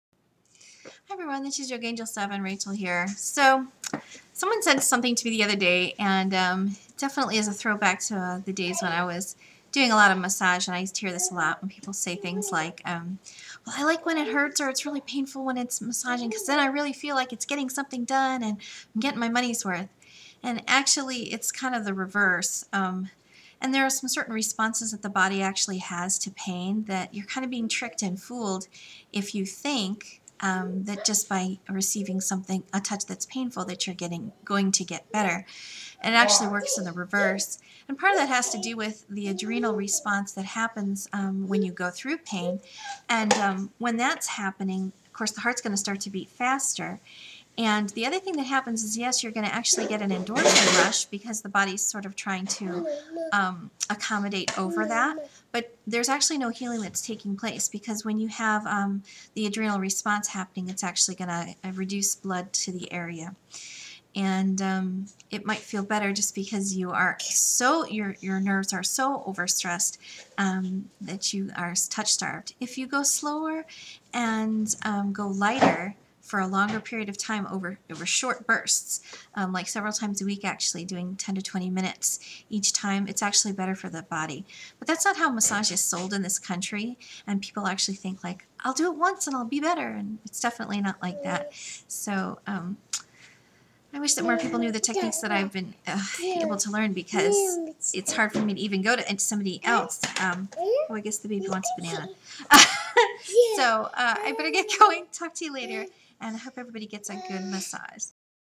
Massage Pain
using techniques which result in pain during massage. Long term results of using techniques which don't stress the body further